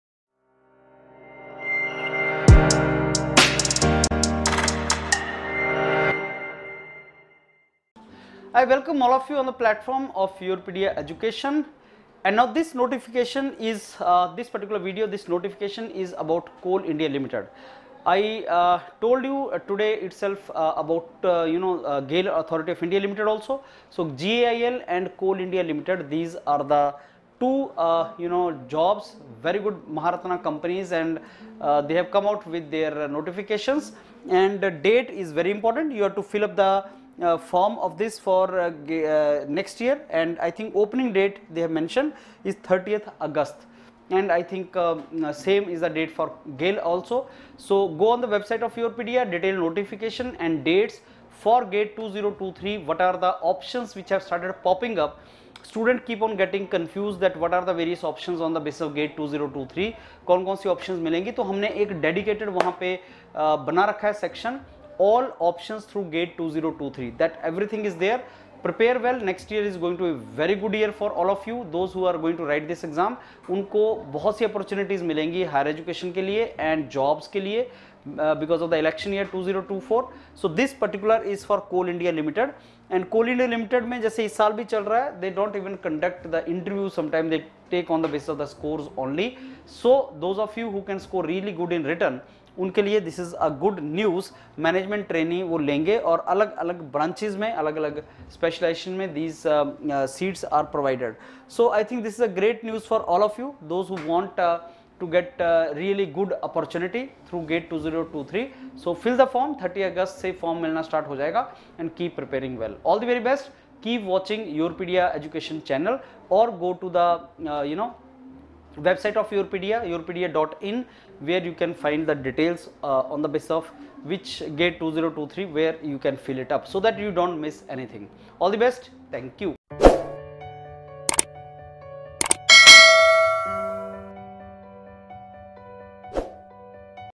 CIL Recruitment 2023 through GATE 2023 Notification out | Prepare CIL Interviews with YourPedia
CIL Recruitment 2023 through GATE 2023 | Prepare CIL Interviews with YourPedia